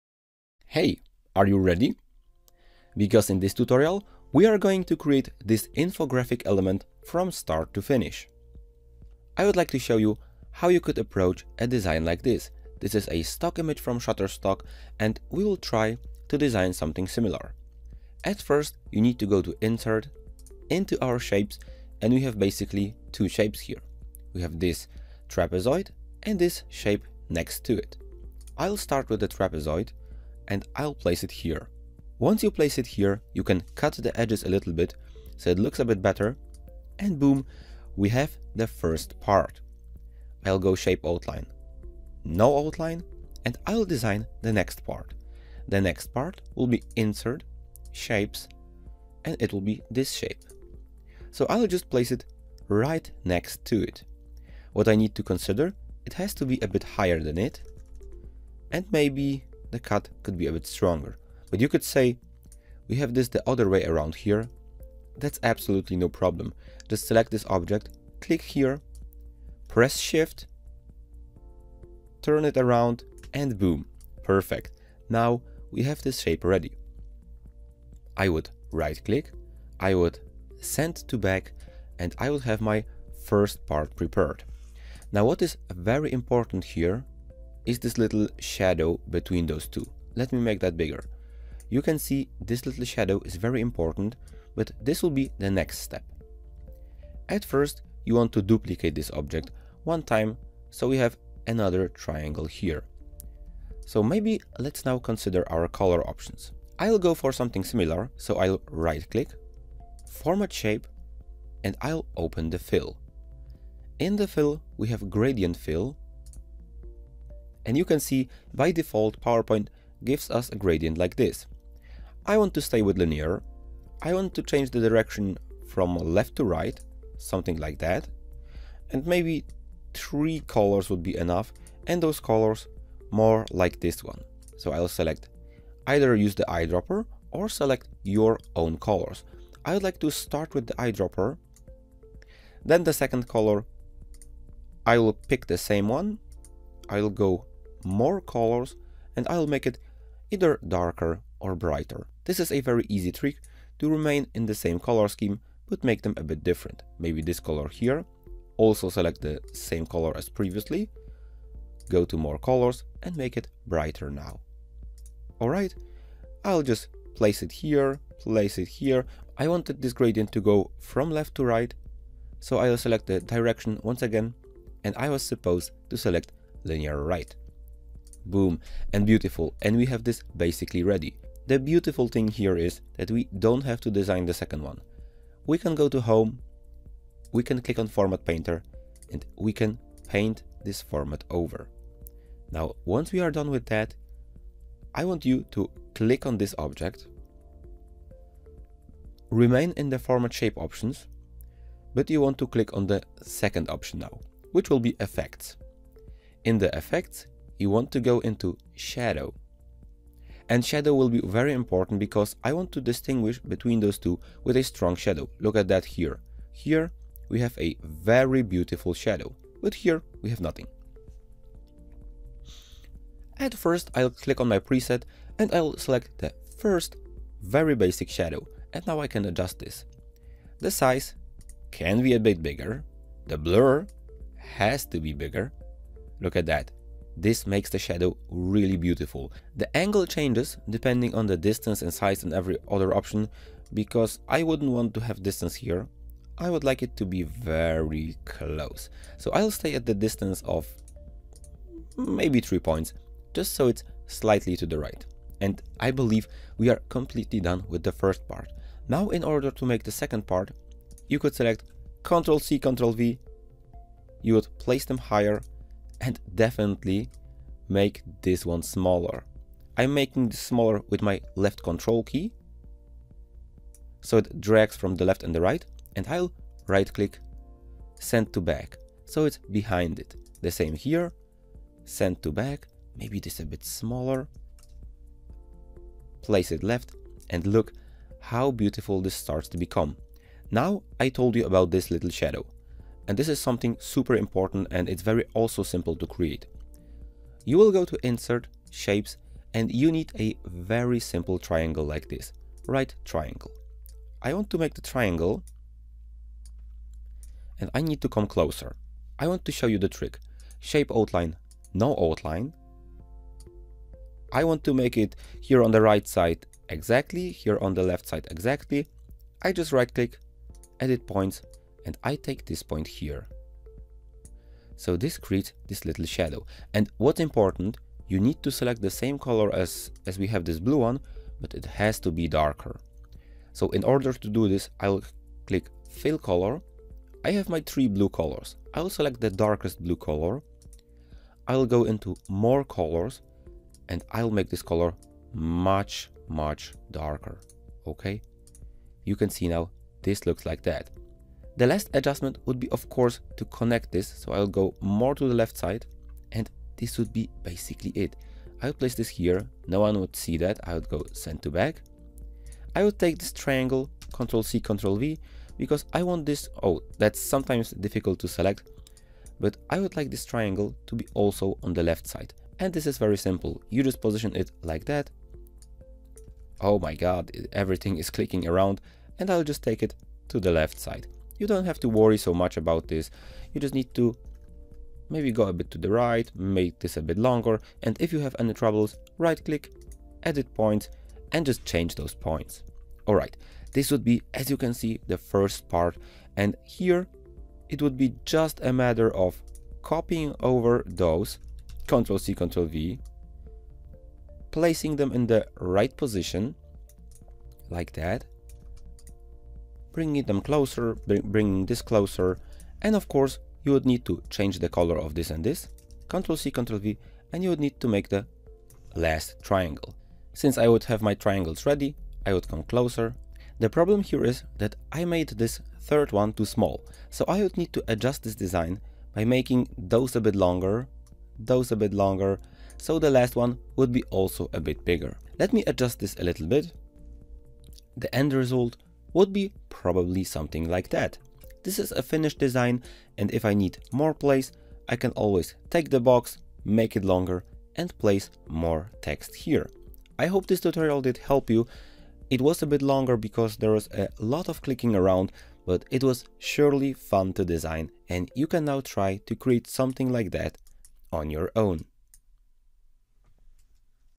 Modern Infographic in PowerPoint - Triangle Chart Design ✔
In this tutorial I would like to go with you about making a modern infographic element - a triangle chart. We will design the entire infographic element together in PowerPoint and add interesting shadows, folds and ribbnos to it. The entire design will be consistent and allow us to select any color scheme to work with. We will be able to display 3 or more relevant informations on one piece.